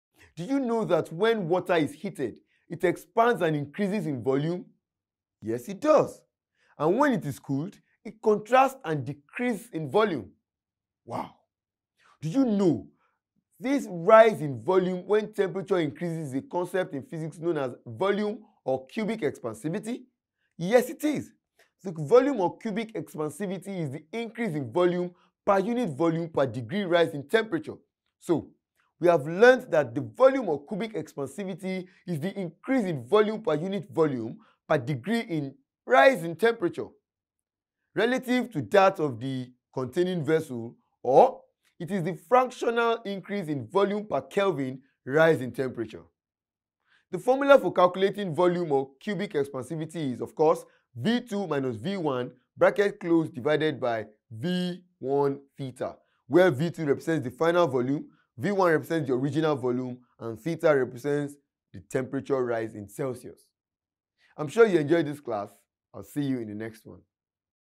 Volume Expansivity | Physics | SSS1
Volume expansivity is the fractional increase in volume of a solid, liquid, or gas per unit rise in temperature, i.e. V2 = V1(1 + γθ), where γ is the cubic expansivity and γ = 3α. ... The absolute expansivity is the apparent expansivity plus the volume expansivity of the container.